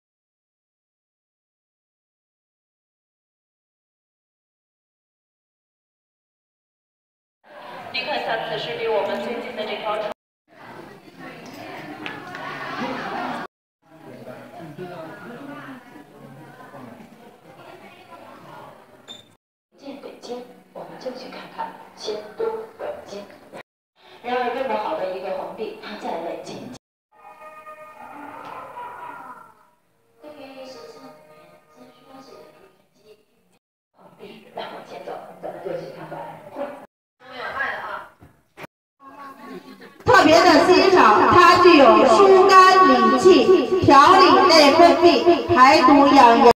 Ming Emporers Wax museum and shopping
Ming Emporers Wax museum and shopping after visiting the Great Wall of China at Badaling